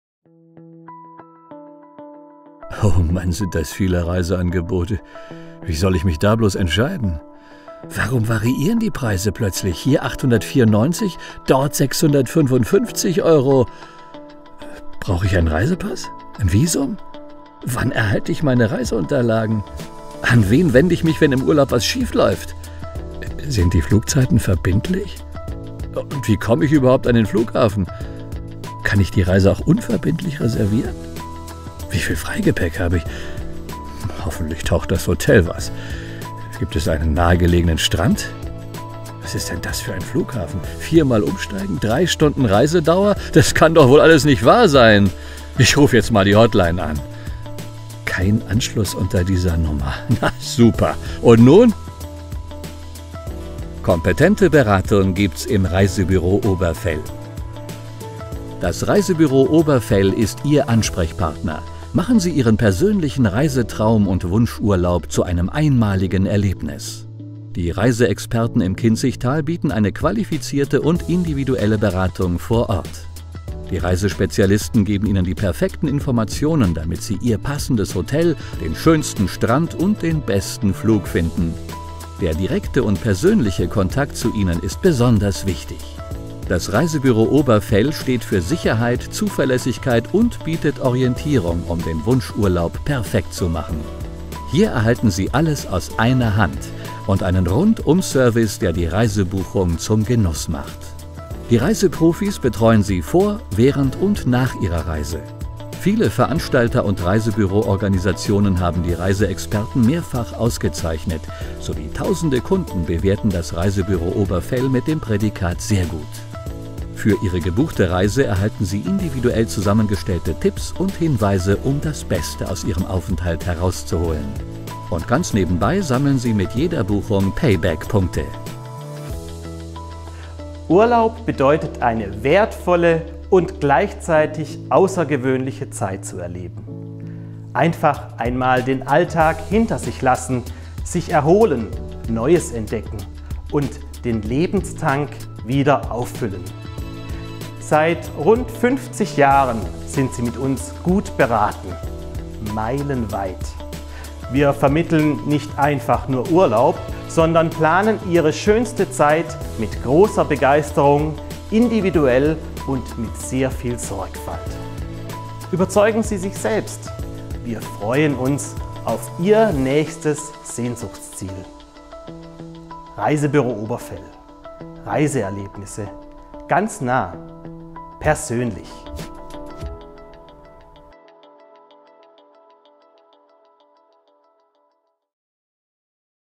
Imagefilm - Reisebüro Oberfell im Kinzigtal (3-Minüter)
Die Reiseexperten vom Reisebüro Oberfell im Kinzigtal bieten eine qualifizierte und individuelle Beratung vor Ort. Wir haben die Vorzüge des Reisebüros festgehalten und in einem Werbefilm verpackt.